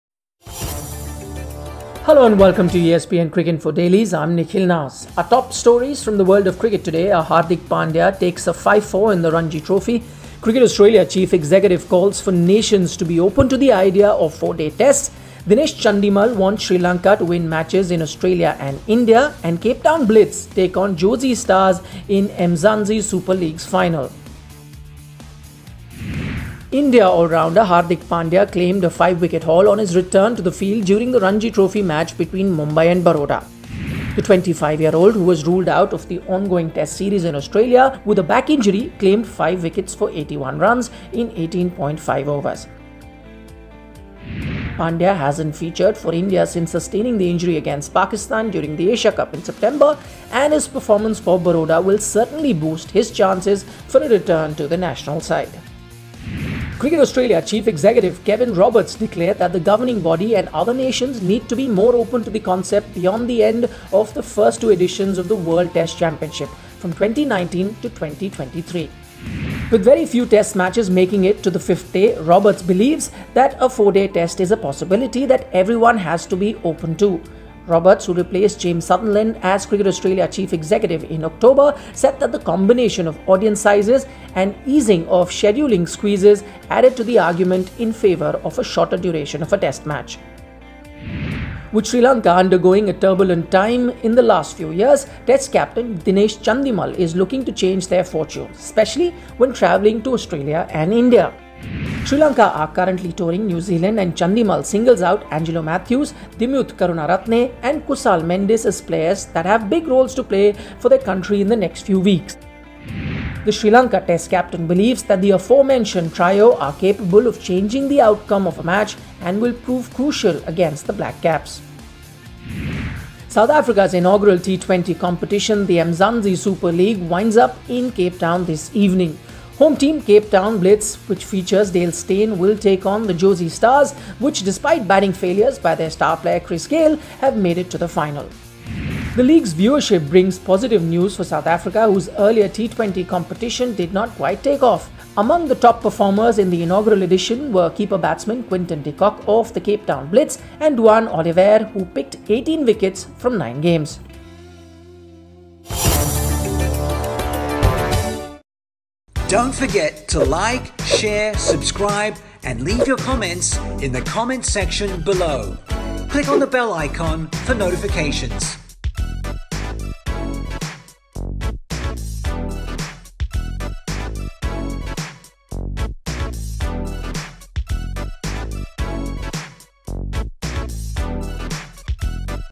Pandya shines on return post injury | Daily Cricket News
Top stories from around the world in our round-up of December 15
1) Hardik Pandya Claims Five-For on Ranji Return
2) Four-Day tests loom in Australia's future
3) Chandimal wants Sri LAnka to win in Australia & India
4) Cape Town blitz take on Jozi stars in MSL Final